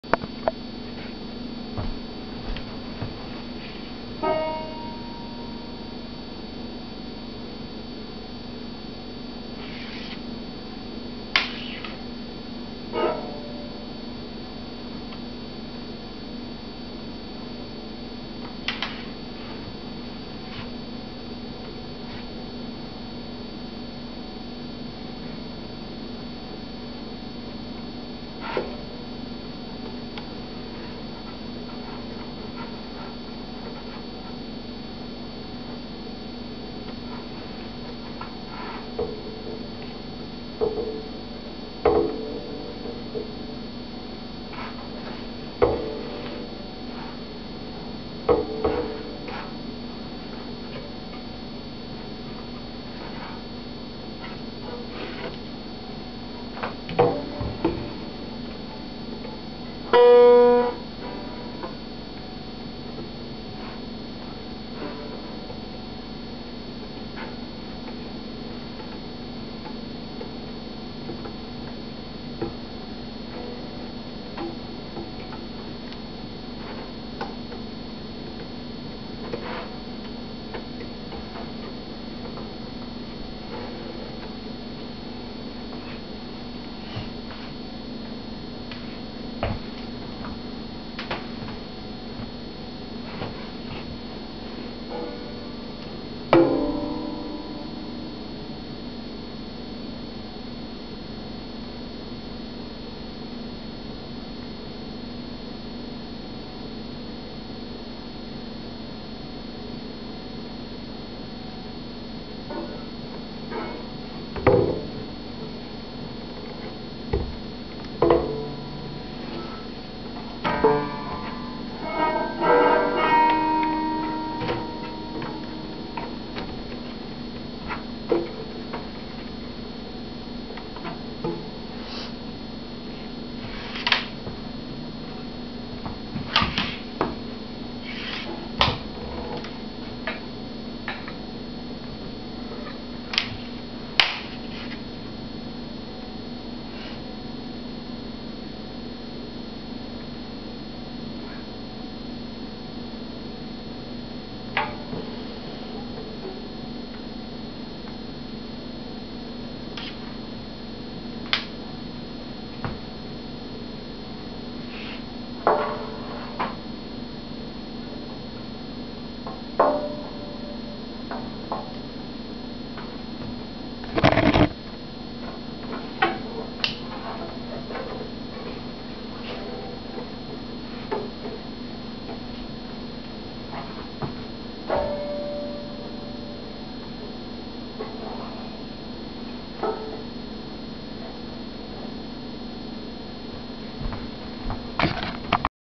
Cradle Banjo Strap Attachment 2
This very boring yet informative video show how our custom made cradle banjo straps attach to a banjo.